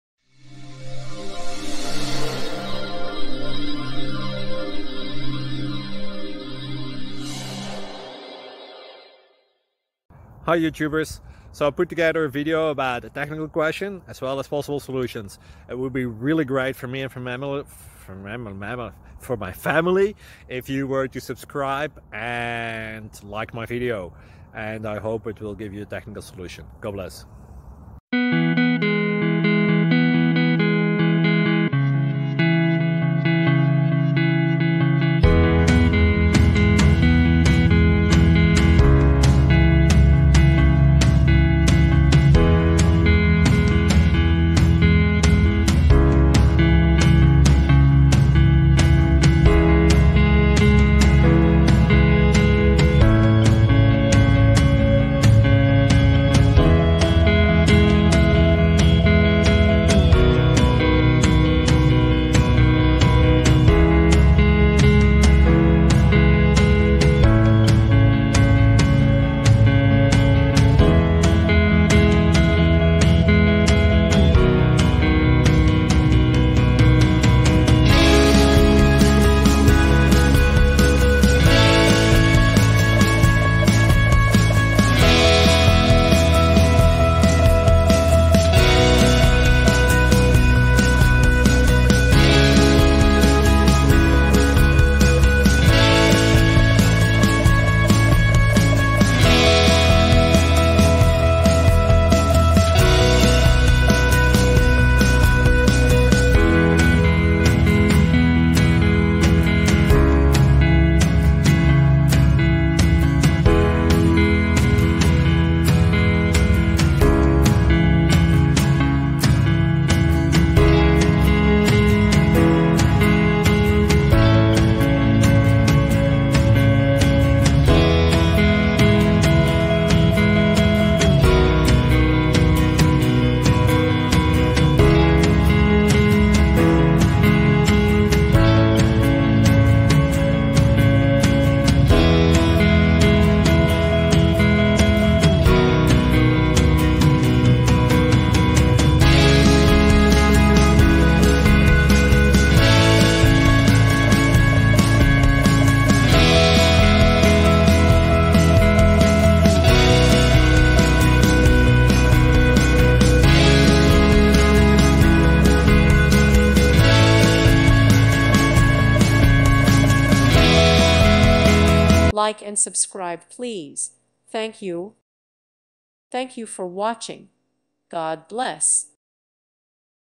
Working around existing MutationObserver to insert DOM elements with Tampermonkey
With thanks & praise to God, and with thanks to the many people who have made this project possible! You are responsible for your own actions.